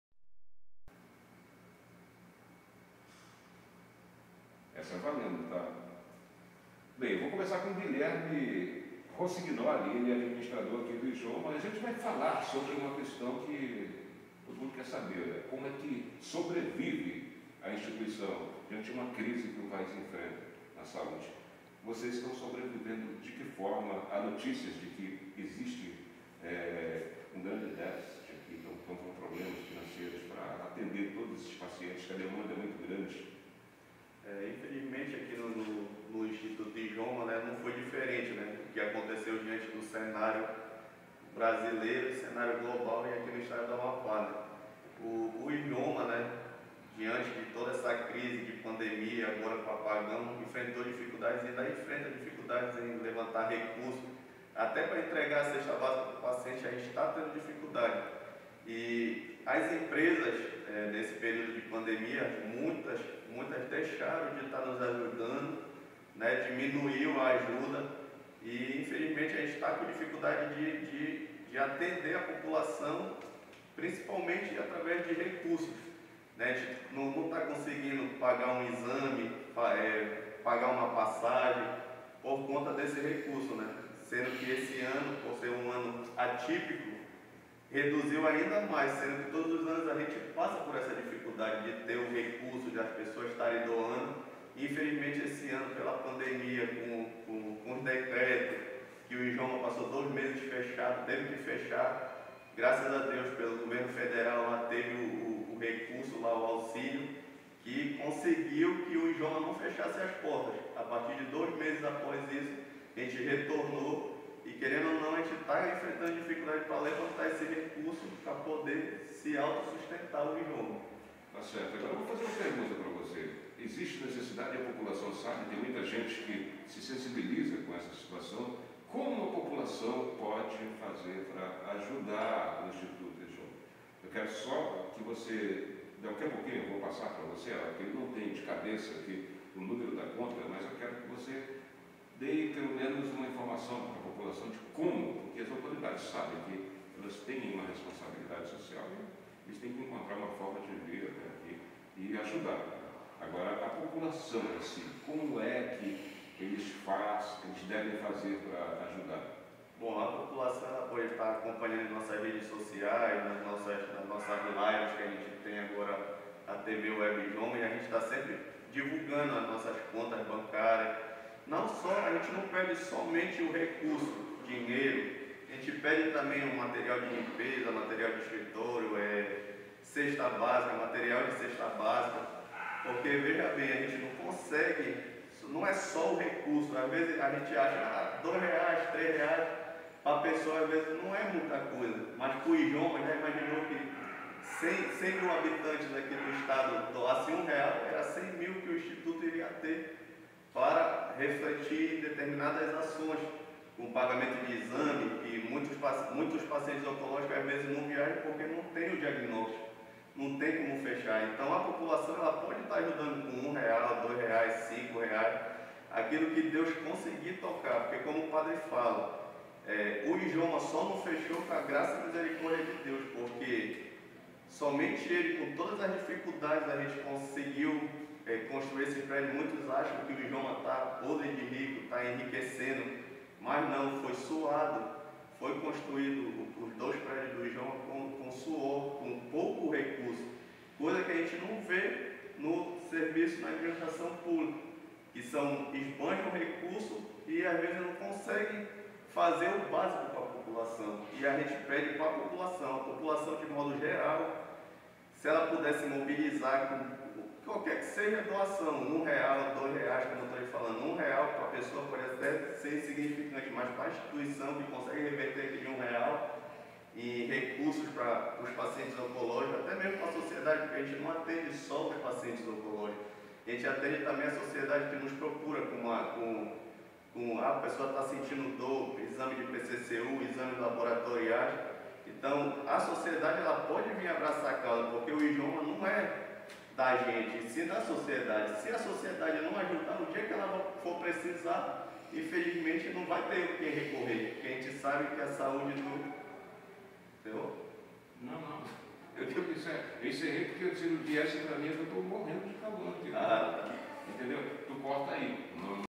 Guilherme Rossignoli (Administrador do Ijoma)
Guilherme Rossignoli fala das demandas da Instituição à TV Cidade, canal 2. Repórter Pimentel Júnior
(Quinta-feira, 03/12/2020)